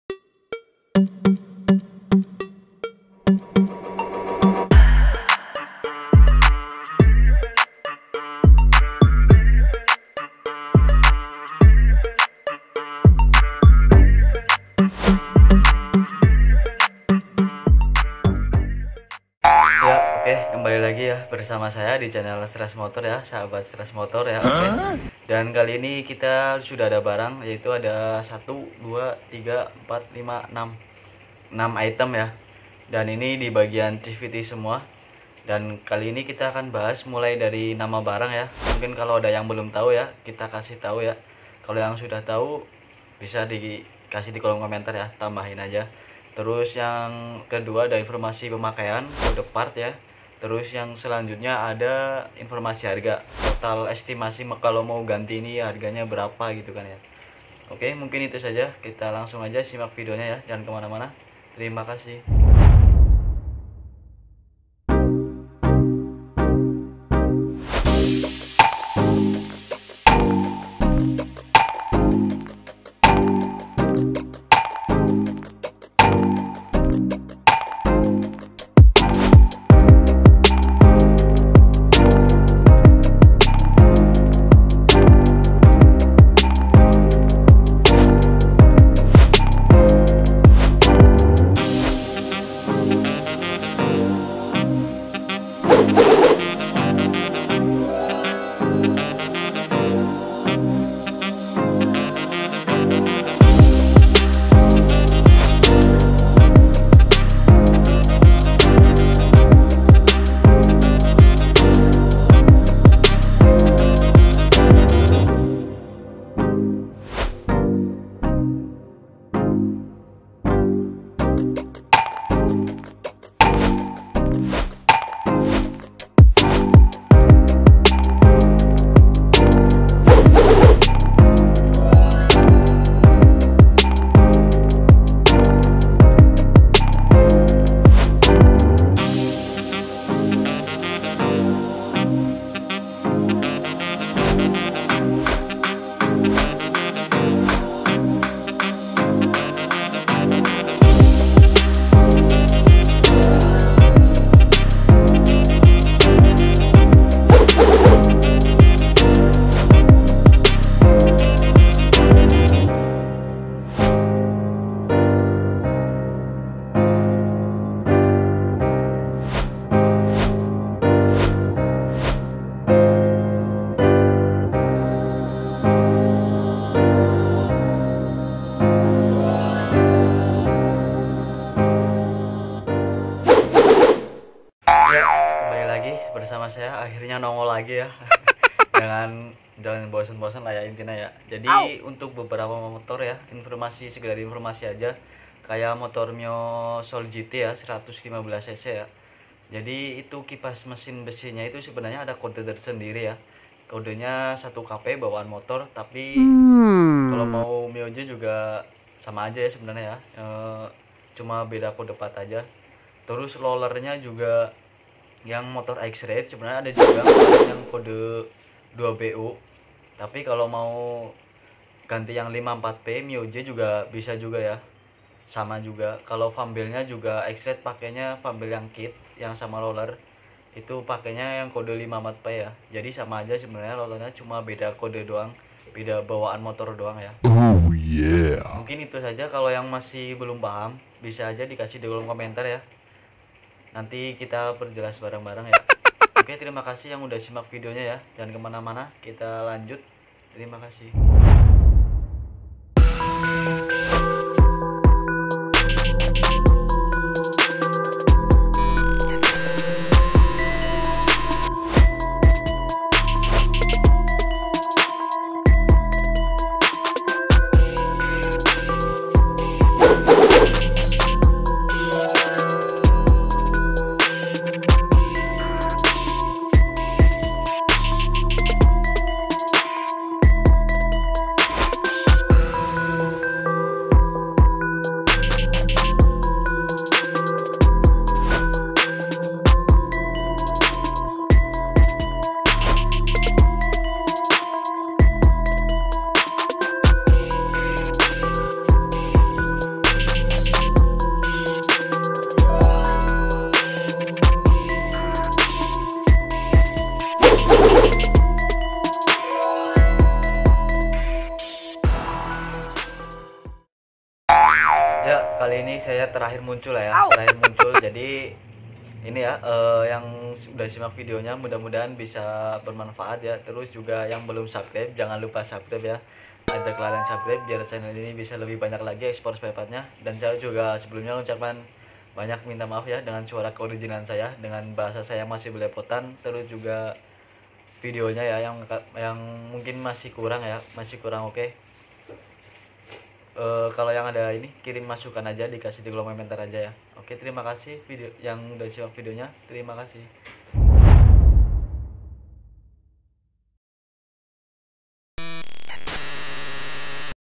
Berikut nama masing masing barang dan kondisi barang kode 54P
Kode 54P
Rumah roller
Kipas mesin besi
Bos rumah roller
Plat Cam
Slider
Roller
Original Yamaha genuine parts